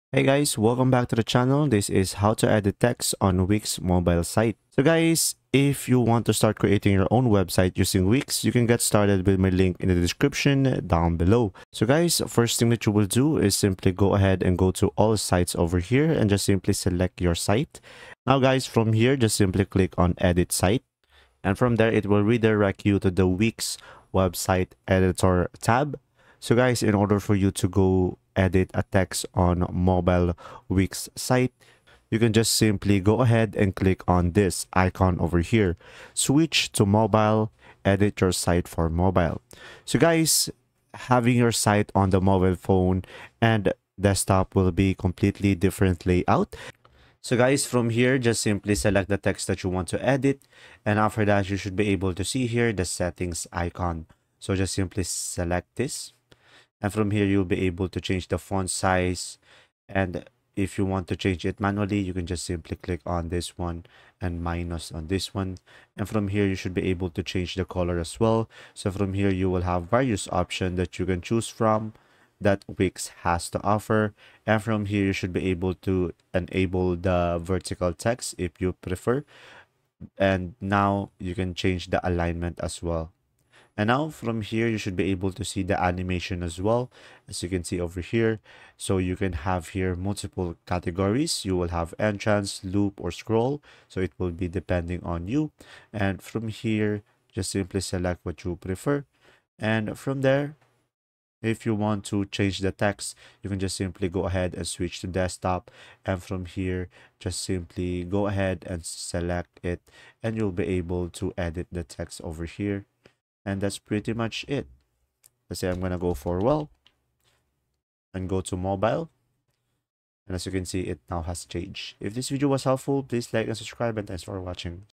How to Edit Text on Wix Mobile Site (EASY GUIDE)
Wonder how to edit text on Wix mobile site? Here's a tutorial on how to edit text on text on Wix mobile site easily.

-- RECOMMENDED VIDEOS --
How To Edit Text On Wix Website 2025 | Edit Text On Wix Mobile Site | Wix Edit Text